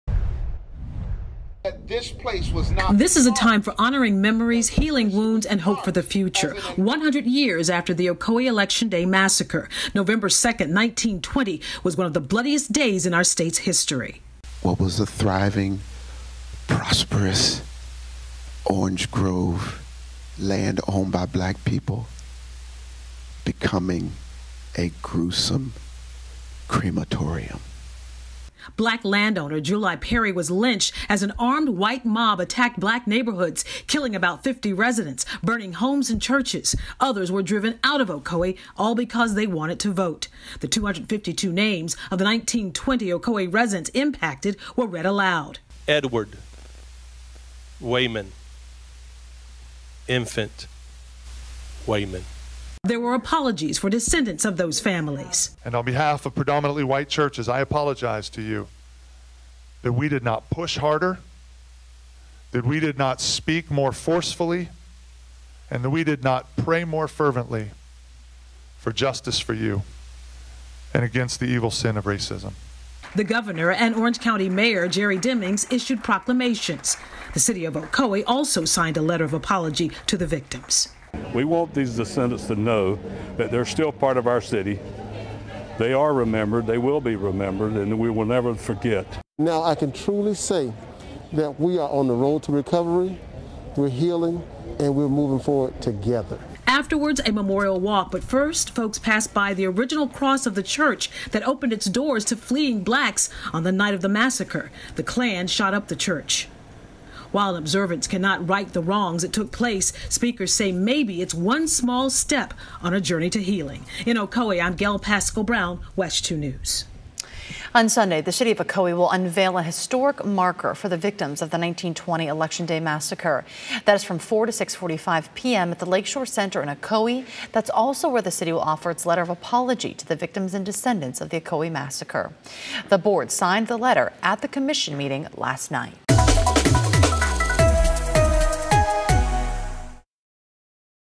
4 day observance of Ocoee Massacre continues with memorial walk
4 day observance of Ocoee Massacre continues with memorial walk